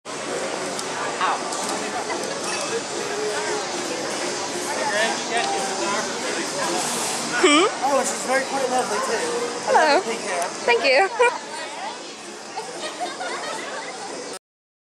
The Merry Misfits Circus side show
While strolling through downtown a week or so ago I ran into some awesome people. more later...
"Freaks, Geeks and SideShow fools! We are a sideshow group that specialized in the odd the strange and the sick"